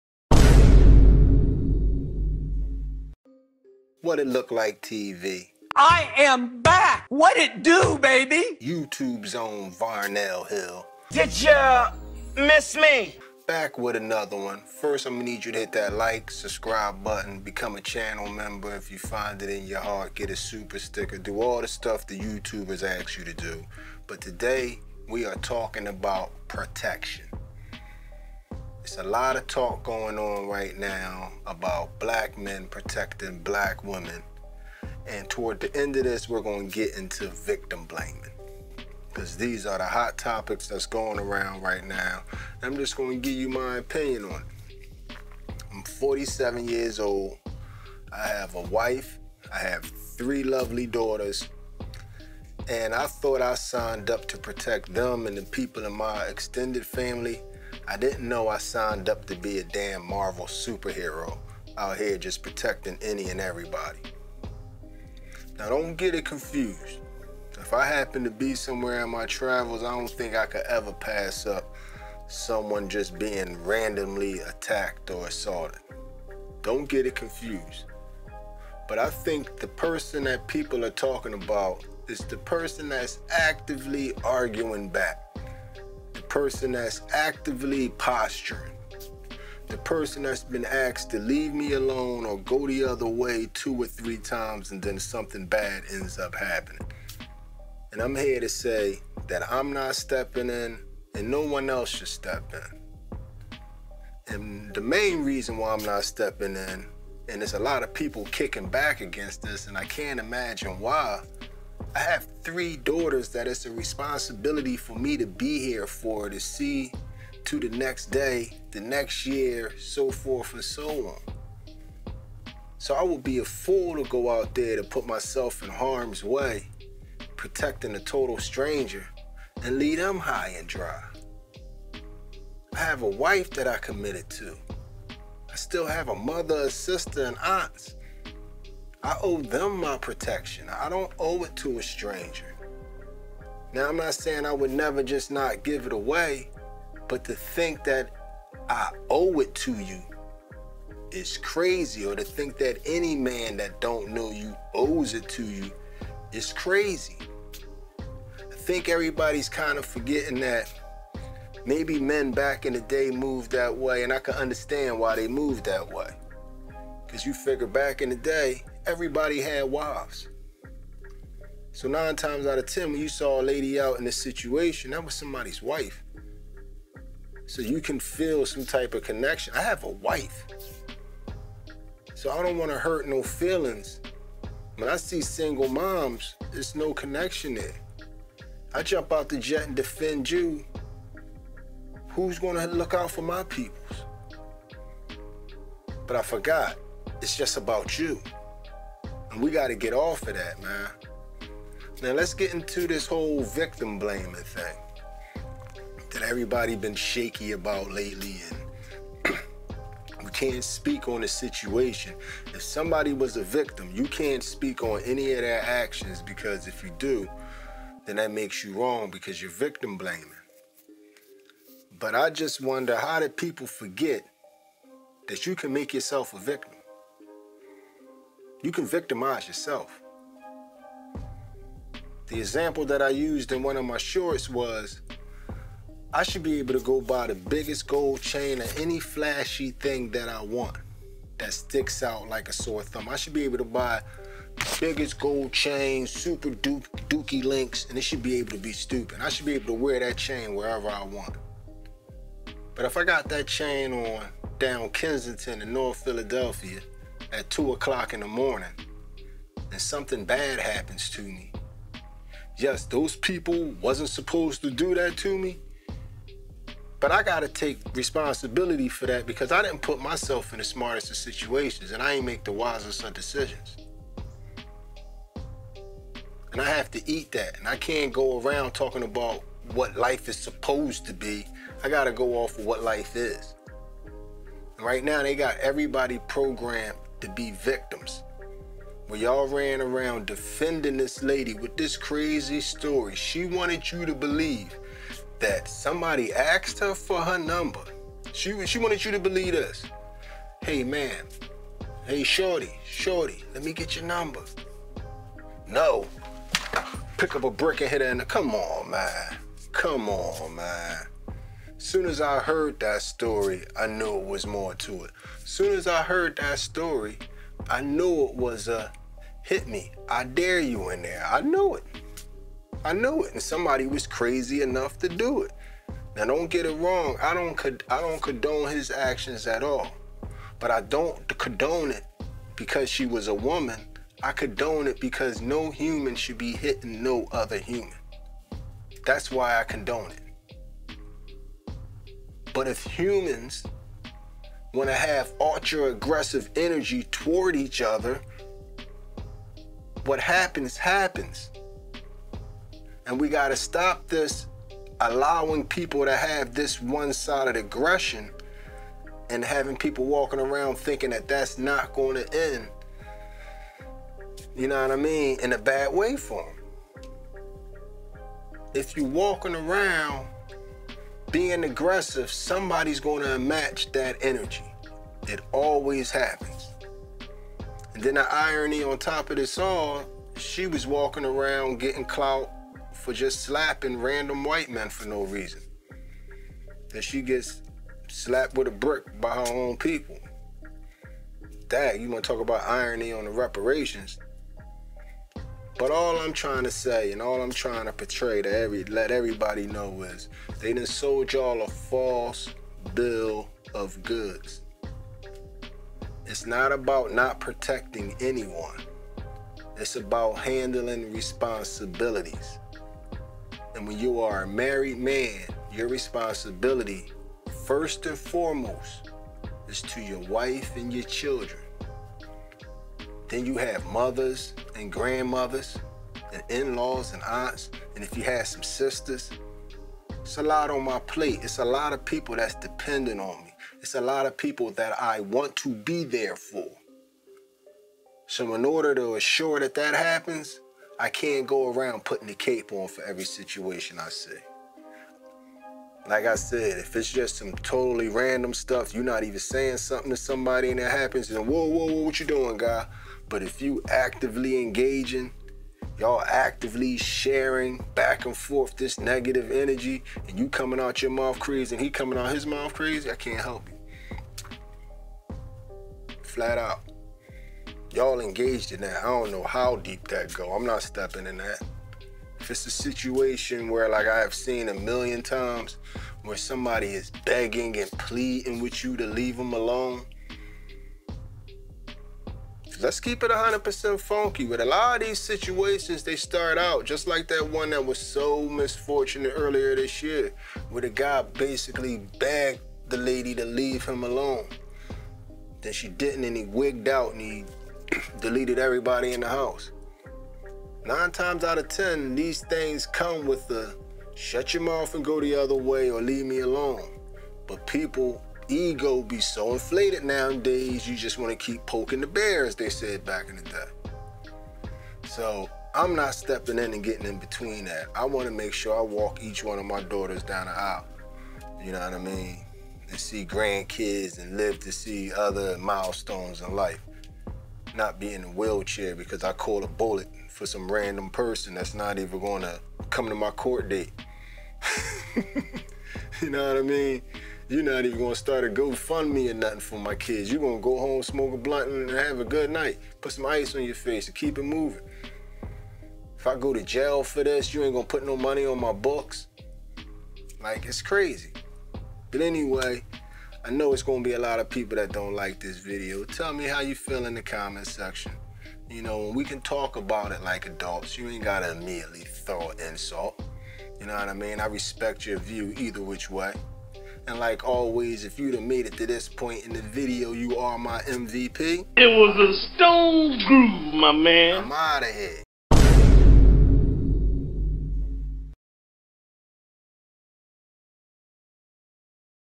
DO MEN OWE ALL WOMEN PROTECTION?
WE TAKE A LOOK AT THE CONVERSATION SURROUNDING MEN PROTECTING WOMEN.